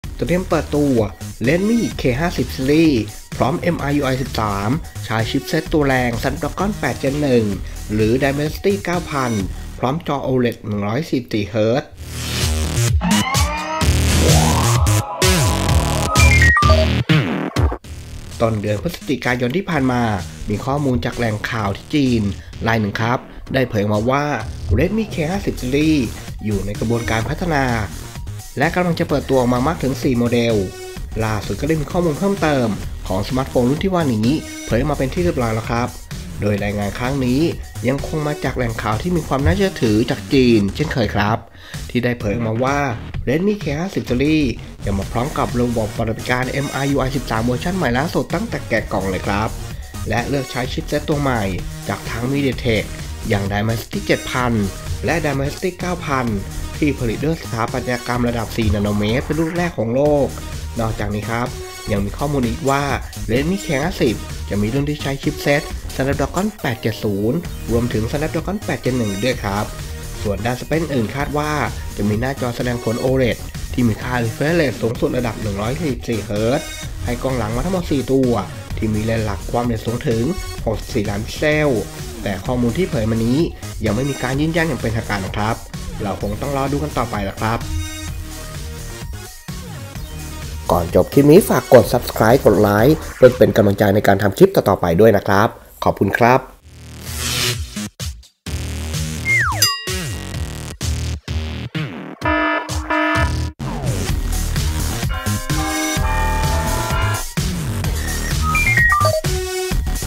เตรียมเปิดตัว Redmi K50 Series พร้อม MIUI 13
เตรียมเปิดตัว Redmi K50 Series พร้อม MIUI 13 ใช้ชิปตัวแรง Snapdragon 8 Gen1/ Dimensity 9000 พร้อมจอ OLED 144Hz